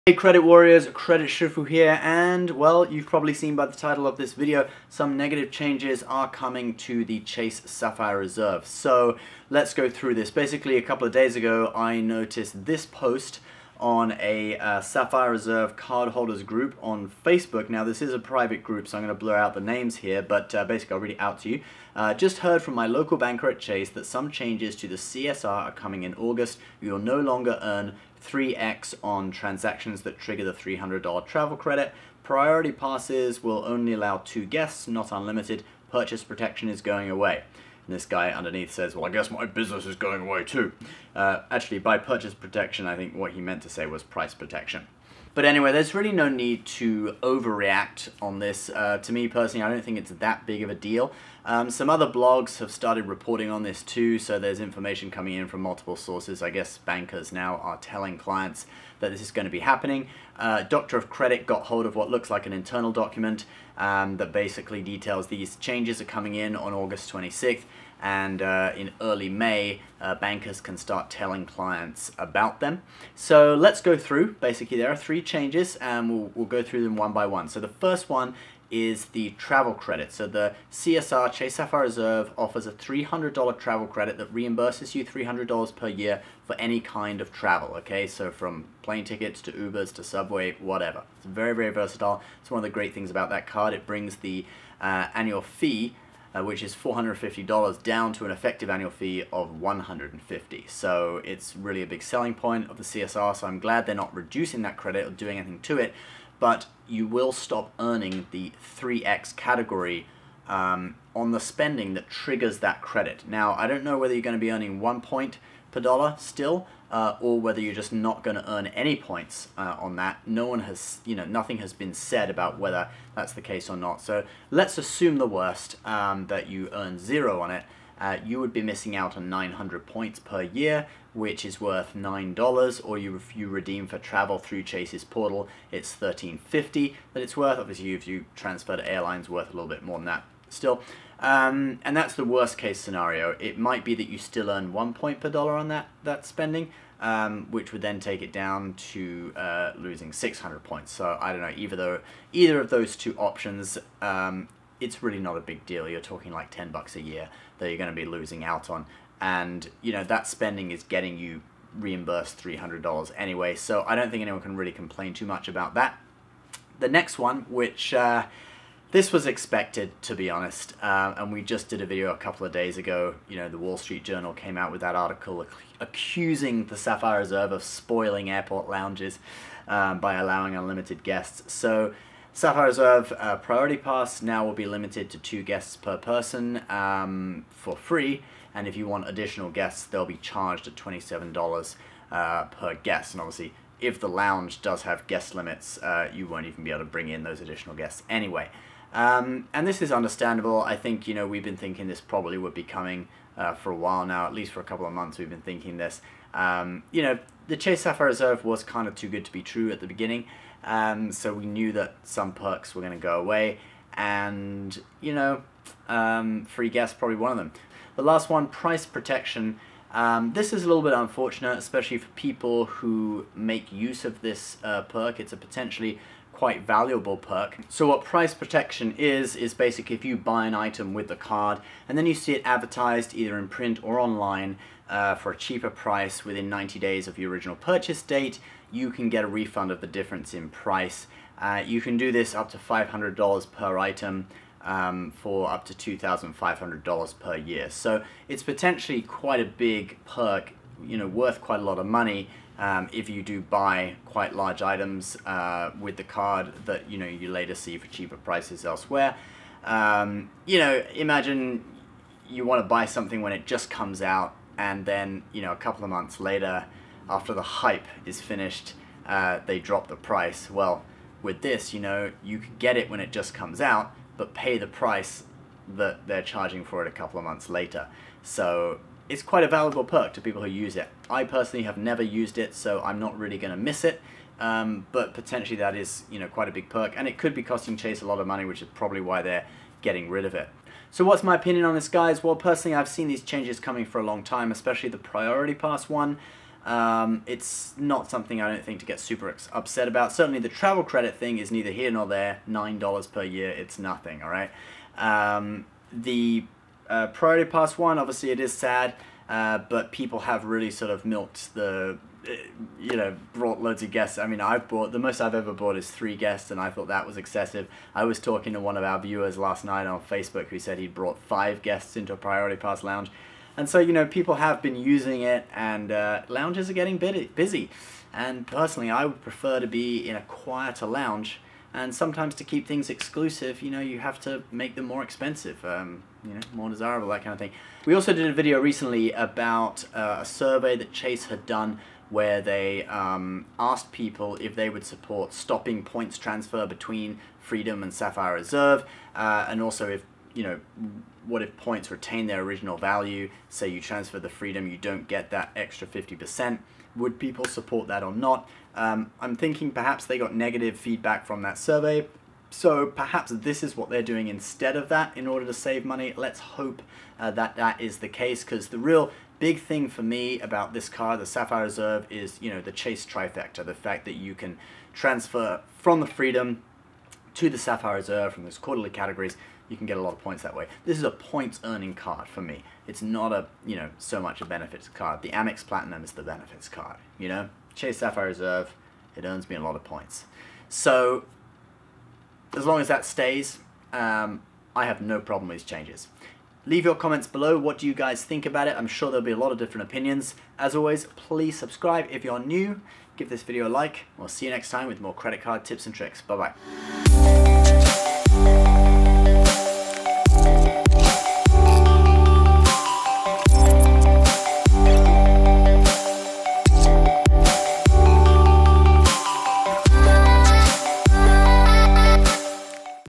Sapphire Reserve: No More Unlimited Lounge Guests, Other Changes Coming
The Chase Sapphire Reserve will no longer allow unlimited guests in Priority Pass lounges, there will also be some other changes.